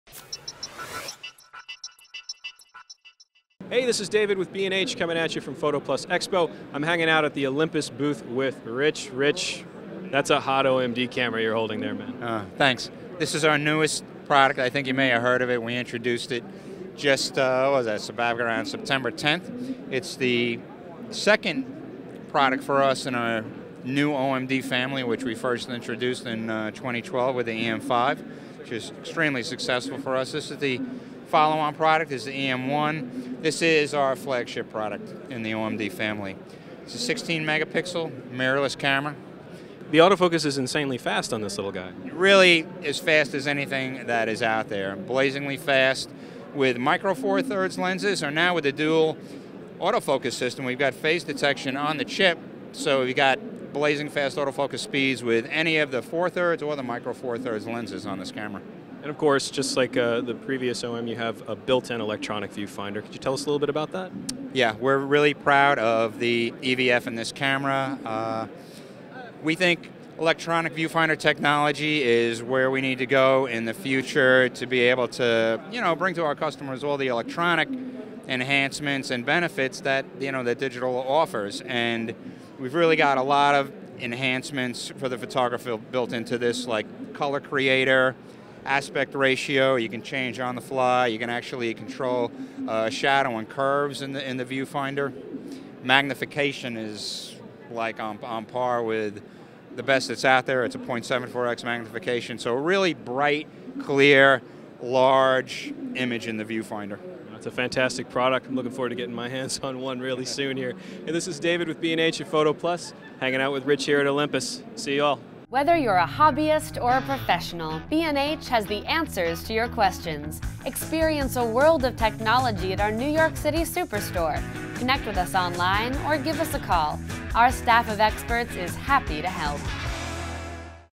PhotoPlus 2013: Olympus
Join us for a chat with Richard Pelkowski from Olympus about the exciting new OM-D E-M1 mirrorless camera. It features a 16.3MP Live MOS Micro Four Thirds sensor, dual-phase and contrast-detection autofocus, a 2,360k-dot electronic viewfinder, and a whole lot more.